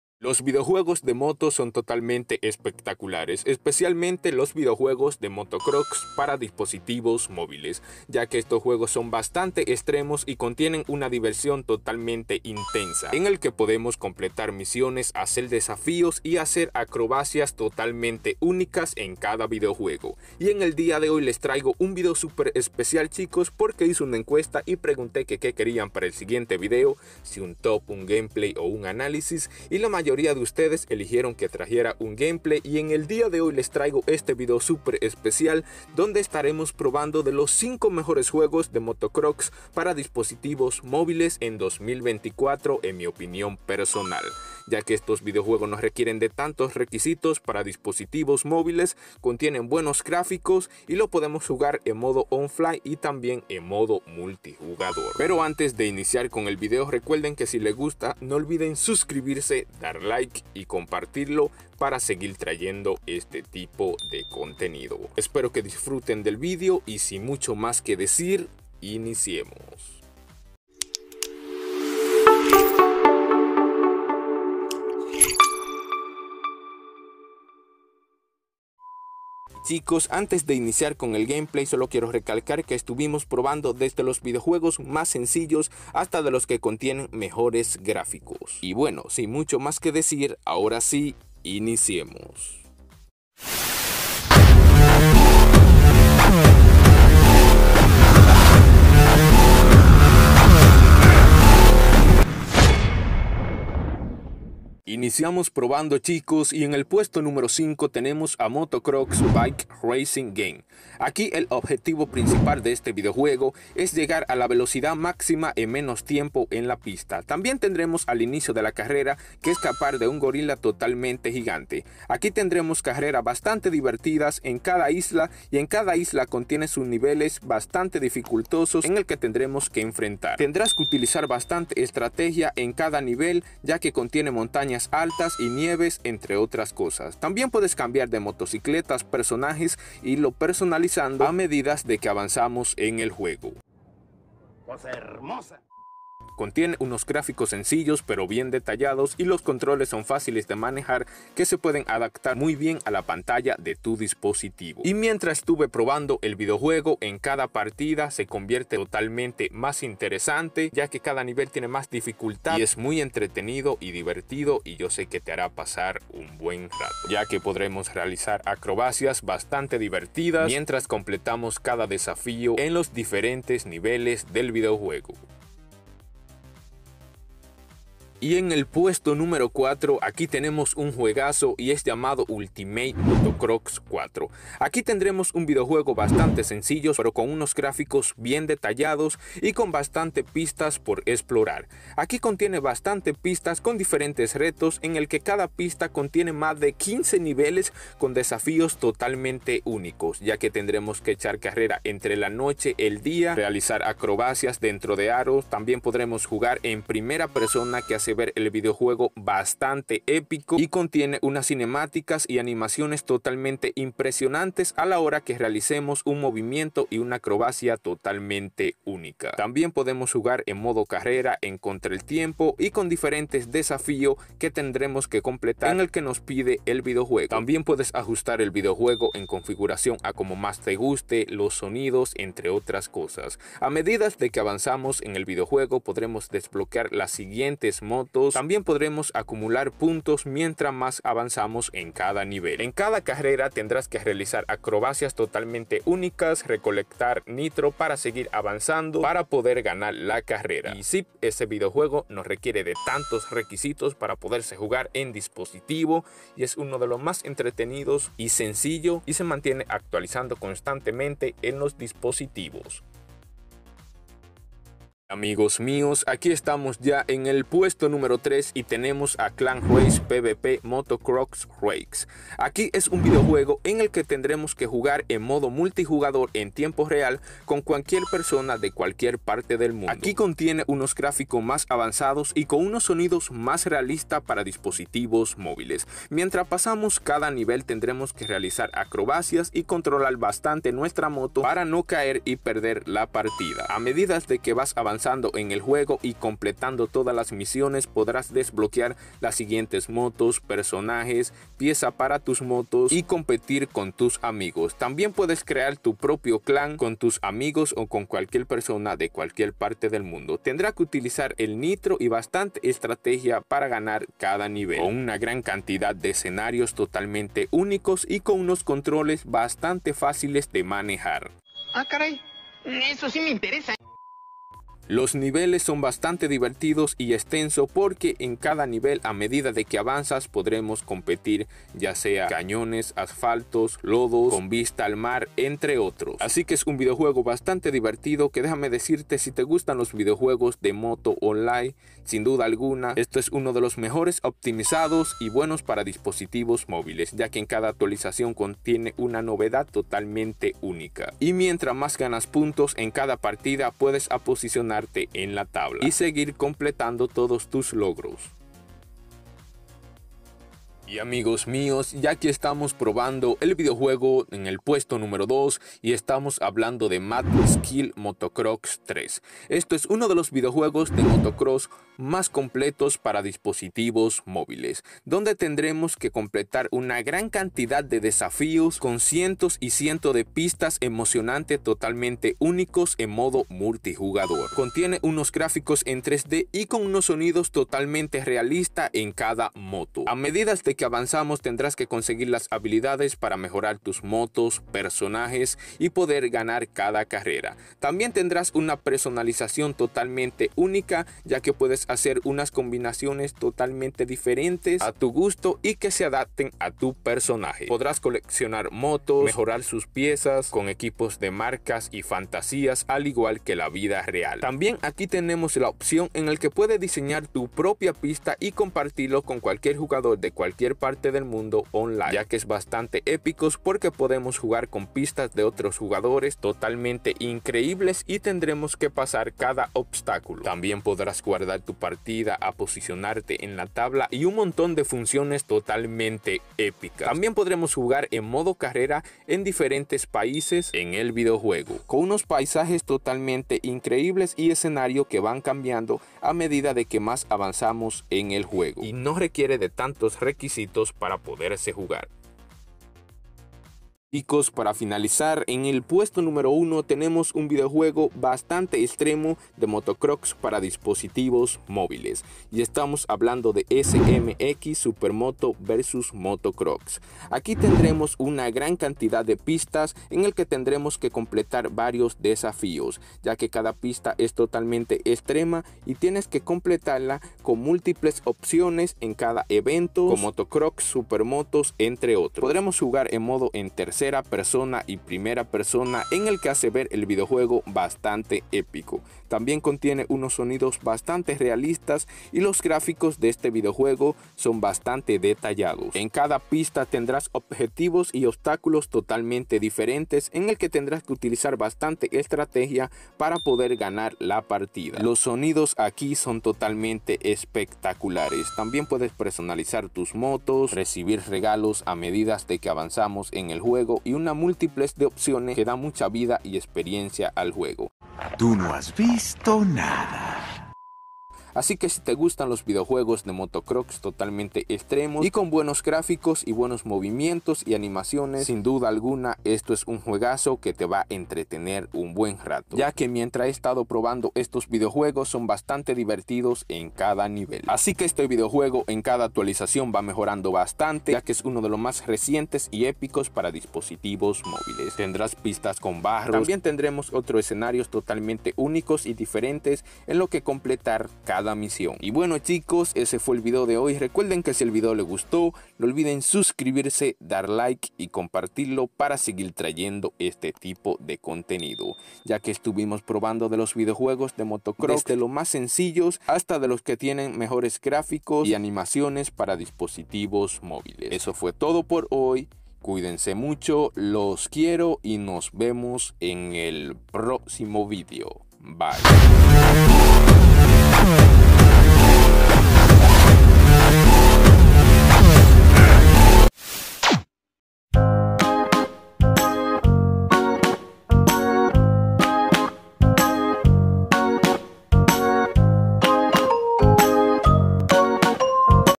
PROBÉ LOS 5 Mejores JUEGAZOS De MOTOCROSS Para ANDROID DE 2024 | ESPECTACULARES 😎
Los Videojuegos De Motos Son Totalmente Espectaculares, Especialmente Los Videojuegos De Motocross Ya Que Son Bastante Divertidos Y Extremos, Ya Que En Ellos Podremos Realizar Misiones Realizar Acrobacias Y Un Montón De Funciones Divertidas, Y En El Dia De Hoy Les Traigo Un Video Super Especial De Un Gameplay Donde Probamos De Los 5 Mejores Juegos De Motocross Para Dispositivos Móviles En 2024 En Mi Opinión Personal, Mientras Jugamos Cada Juego Los Analizamos Y Hablamos De Sus Grandes Carateristicas, Espero Que Disfruten Del Vídeo Los Quiero :'D

Y Chicos\as Como Siempre Los Quiero Y Nos Vemos En El Próximo Video Bye ❤.